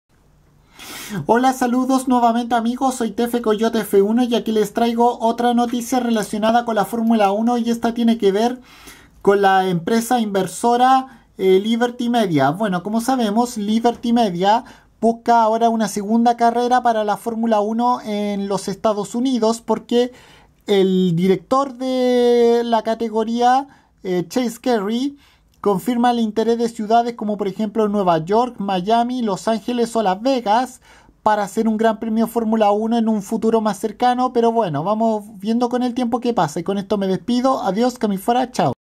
Liberty Media busca un segundo GP de F1 en Estados Unidos
Ciudades como Miami,Las Vegas,Nueva York o Los Ángeles confirman el interes según Chase Carey